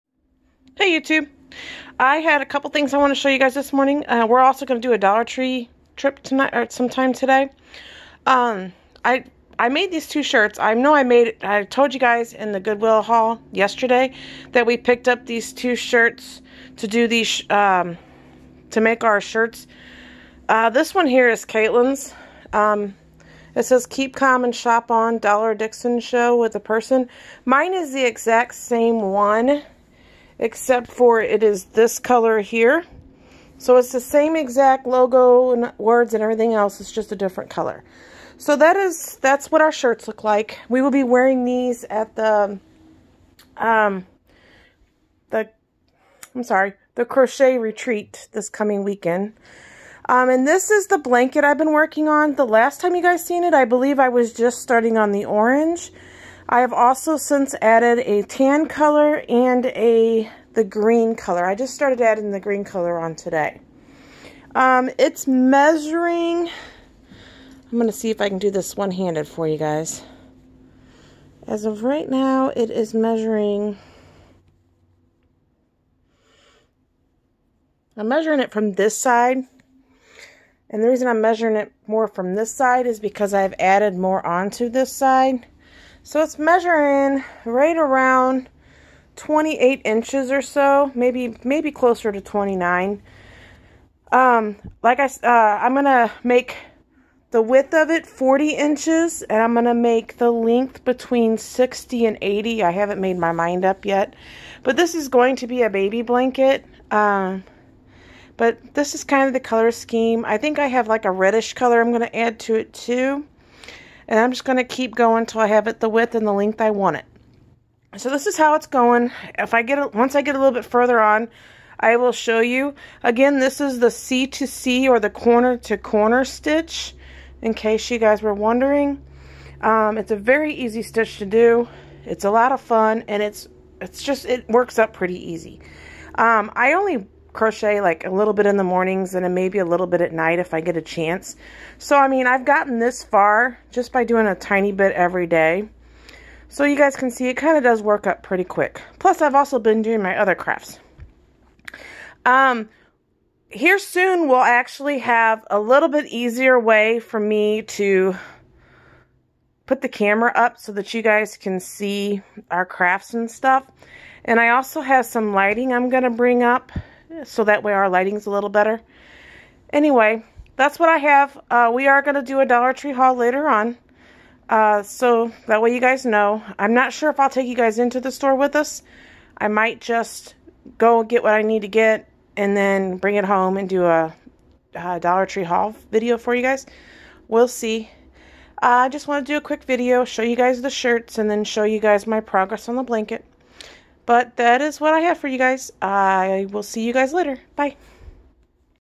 Channel tshirts and progress on the c2c blanket
C2c crochet, channel merch,